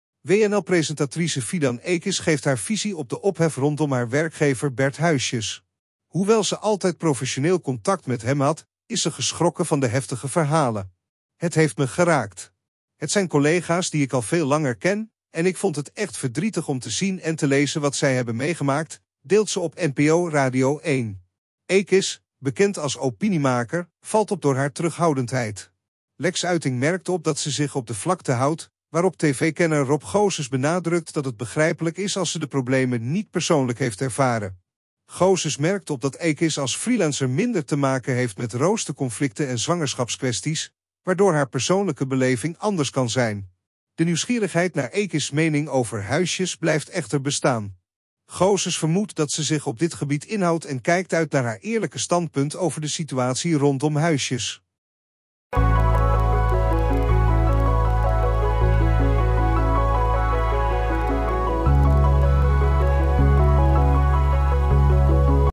Fidan Ekiz doorbreekt stilte rond mediarel: 'Opvallend dat ze zich inhoudt!
WNL-presentatrice Fidan Ekiz geeft haar visie op de ophef rondom haar werkgever Bert Huisjes. Hoewel ze altijd professioneel contact met hem had, is ze geschrokken van de heftige verhalen. “Het heeft me geraakt. Het zijn collega’s die ik al veel langer ken, en ik vond het echt verdrietig om te zien en te lezen wat zij hebben meegemaakt,” deelt ze op NPO Radio 1.
Ekiz, bekend als opiniemaker, valt op door haar terughoudendheid. Lex Uiting merkt op dat ze zich op de vlakte houdt, waarop tv-kenner Rob Goossens benadrukt dat het begrijpelijk is als ze de problemen niet persoonlijk heeft ervaren. Goossens merkt op dat Ekiz als freelancer minder te maken heeft met roosterconflicten en zwangerschapskwesties, waardoor haar persoonlijke beleving anders kan zijn.
De nieuwsgierigheid naar Ekiz's mening over Huisjes blijft echter bestaan. Goossens vermoedt dat ze zich op dit gebied inhoudt en kijkt uit naar haar eerlijke standpunt over de situatie rondom Huisjes.